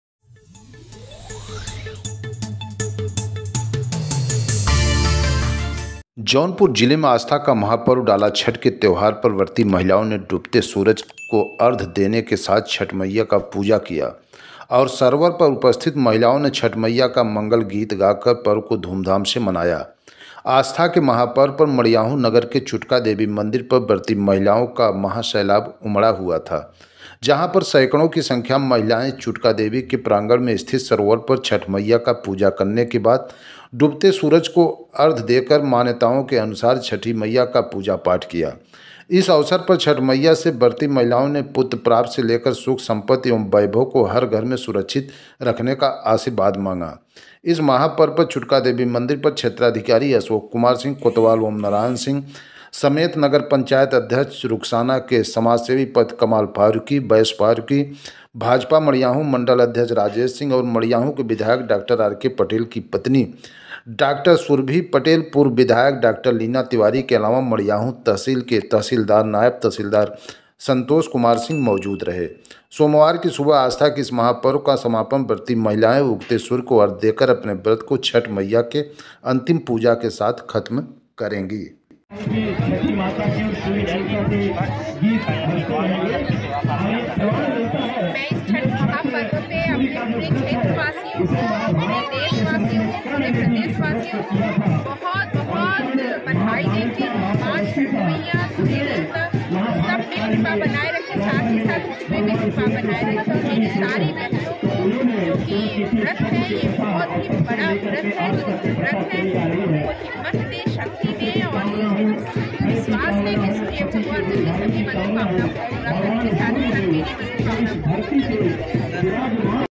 आस्था का महापर्व डाला छठ पर व्रती महिलाओं ने डूबते सूरज को अर्घ देने के साथ छठ मईया का पूजा किया
जौनपुर जिले में आस्था का महापर्व डाला छठ के त्यौहार पर व्रती महिलाओं ने डूबते सूरज को अर्घ देने के साथ छठ मईया का पूजा किया और सरवर पर उपस्थित महिलाओं ने छठ मैया का मंगल गीत गाकर पर्व को धूमधाम से मनाया। आस्था के महापर्व पर मड़ियाहूं नगर के चुटका देवी मंदिर पर व्रती महिलाओं का महासैलाब उमड़ा हुआ था। जहां पर सैकड़ों की संख्या में महिलाएं चुटका देवी के प्रांगण में स्थित सरोवर पर छठ मैया का पूजा करने के बाद डूबते सूरज को अर्थ देकर मान्यताओं के अनुसार छठी मैया का पूजा पाठ किया। इस अवसर पर छठ मैया से व्रति महिलाओं ने पुत्र प्राप्त से लेकर सुख संपत्ति एवं वैभव को हर घर में सुरक्षित रखने का आशीर्वाद मांगा। इस महापर्व पर चुटका देवी मंदिर पर क्षेत्राधिकारी अशोक कुमार सिंह कोतवाल ओम नारायण सिंह समेत नगर पंचायत अध्यक्ष रुखसाना के समाजसेवी पति कमाल फारुकी, वैश्य फारुकी, भाजपा मड़ियाहूं मंडल अध्यक्ष राजेश सिंह, और मड़ियाहूं के विधायक डॉक्टर आरके पटेल की पत्नी डॉ सुरभि पटेल पूर्व विधायक डॉ लीना तिवारी के अलावा मड़ियाहूं तहसील के तहसीलदार, नायब तहसीलदार संतोष कुमार सिंह मौजूद रहे। सोमवार की सुबह आस्था के इस पर्व का समापन व्रती महिलाएं उगते सूर्य को अर्घ्य देकर अपने व्रत को छठ मैया के अंतिम पूजा के साथ खत्म करेंगी।